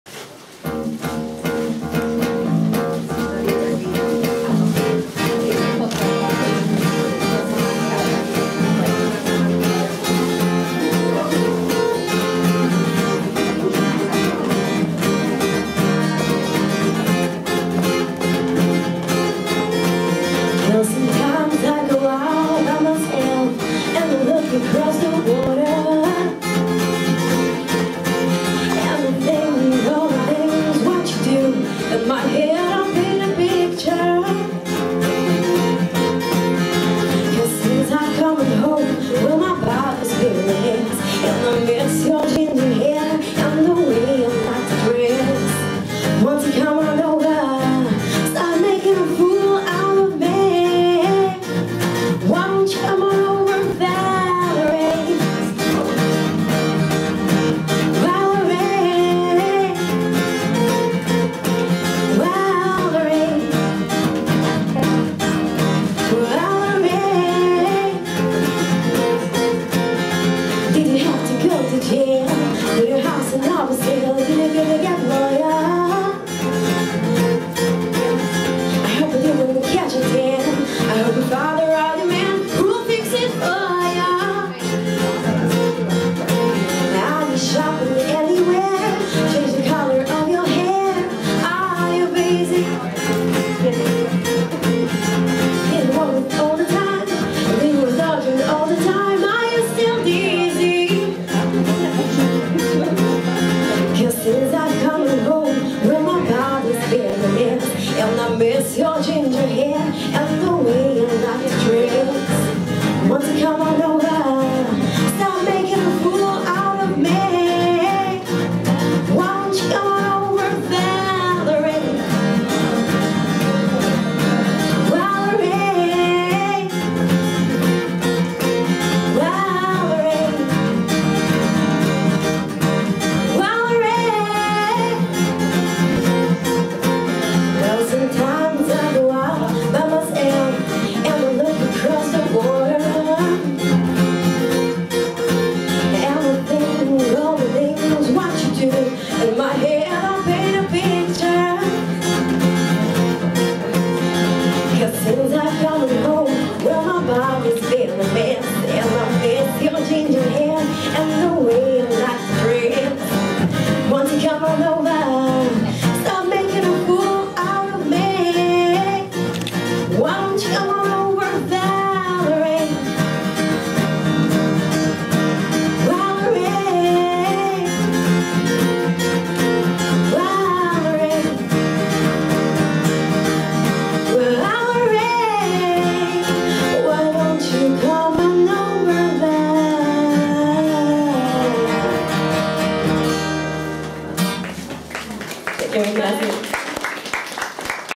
L. U. S. K - (Výstava dvorné dámy 2016)
Počas otvorenia  výstavy Dvorné dámy zahrala a zaspievali členovia skupiny L. U. S. K